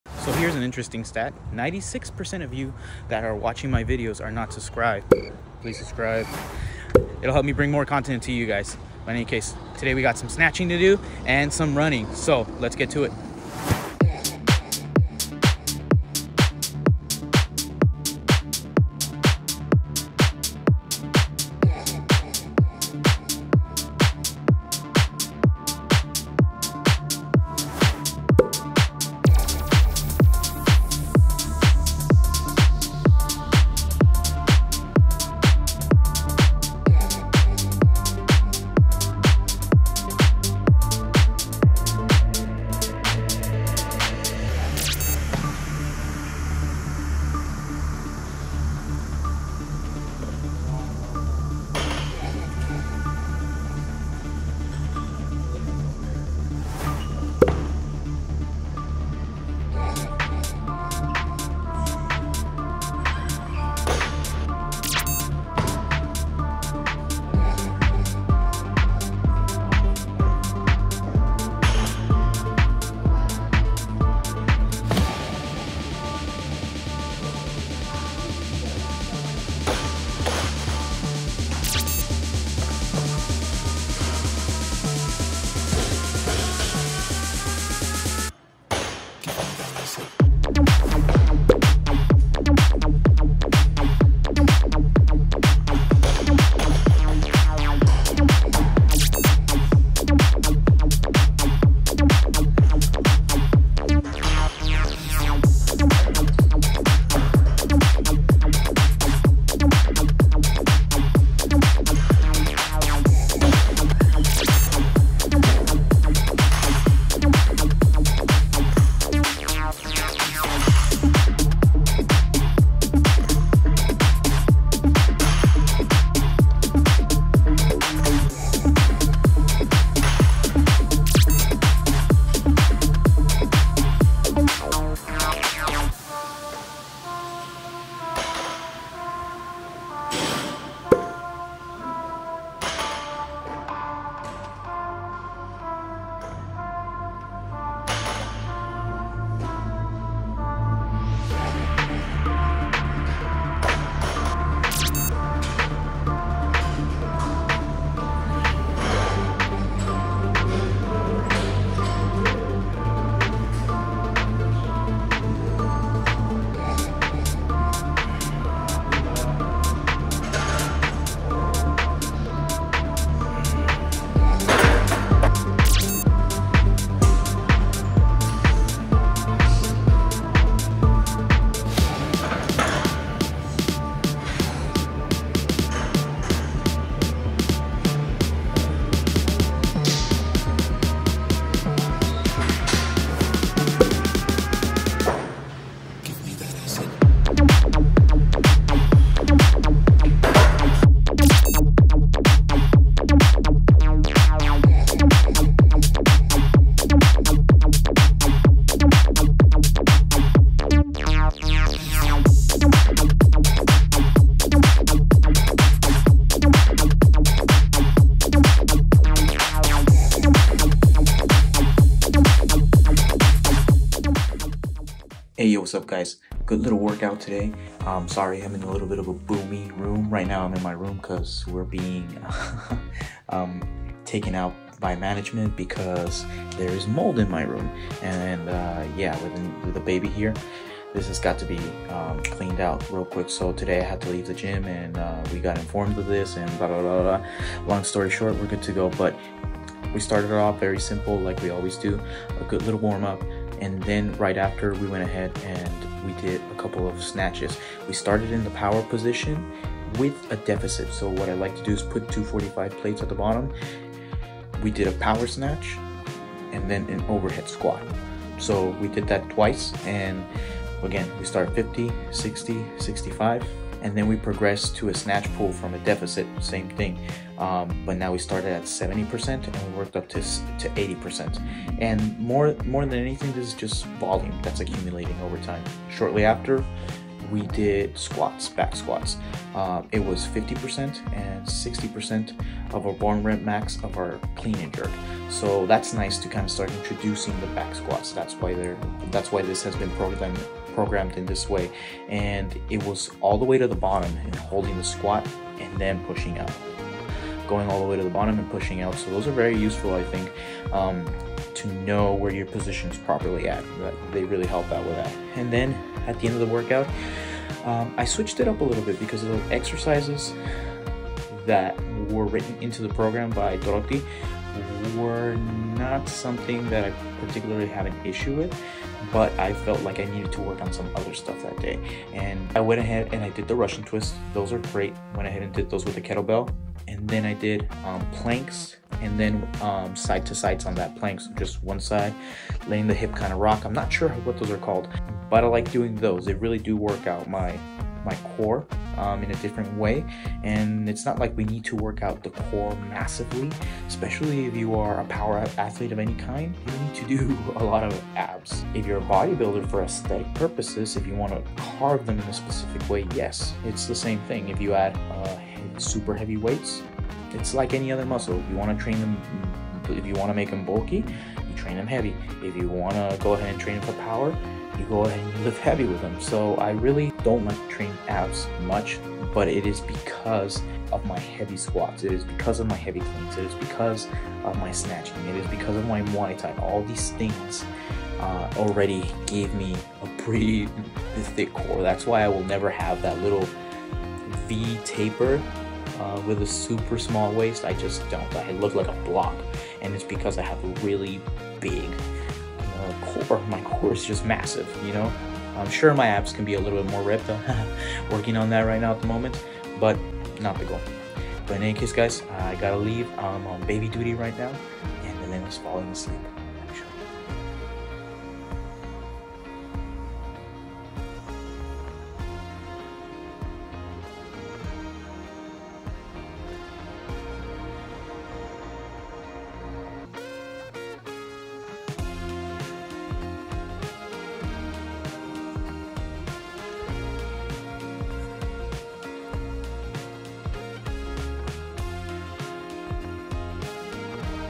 SNATCHING DAY!
TODAY WE ARE GOING TO SNATCH BUT WITH A SLIGHT TWIST. WE WILL BE USING A DEFICIT TO HELP US PREP OUR LOWER BAC AND BETTER HELP US UNDERSTAND OUR POSITION WHILE SNATCHING. ALSO, I HAVE A MOLD PROBLEM AT HOME 😅 WITH OUR NEW BORN THIS IS NO GO SO WAIT TO THE END AND YOU MIGHT SEE LITTLE ELENA GROWING!

ZAID OUT!